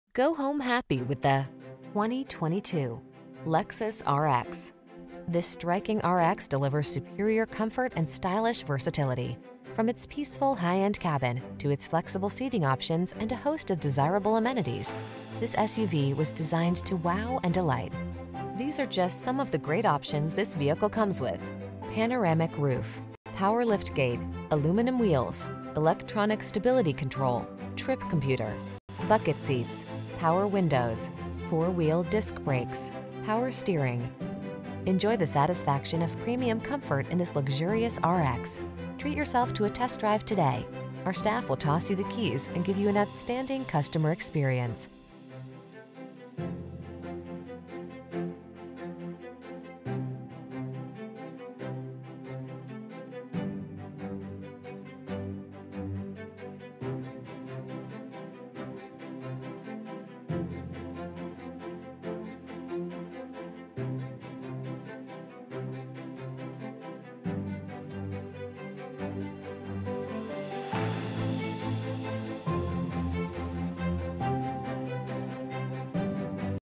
2022 Lexus RX Lexington, Wyandotte, Colby, Becknerville, Clintonville, KY X30124
Eminent White Pearl New 2022 Lexus RX available in Lexington, KY at Lexus of Lexington. Servicing the Lexington, Wyandotte, Colby, Becknerville, Clintonville, KY area.

1264 E New Circle Road